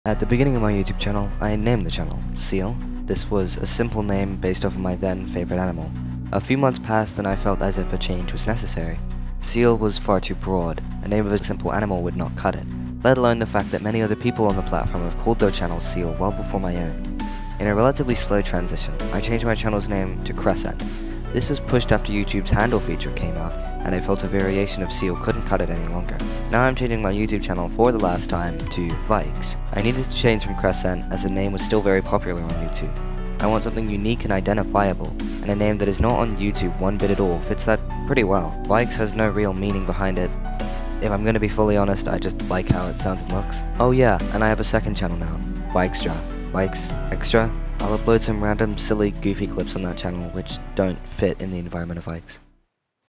Why Vyxse?
In this video I explain the motivation behind the channel name change to "Vyxse" & also introduce the second channel where I will post silly little videos.

Hours to make this video: 1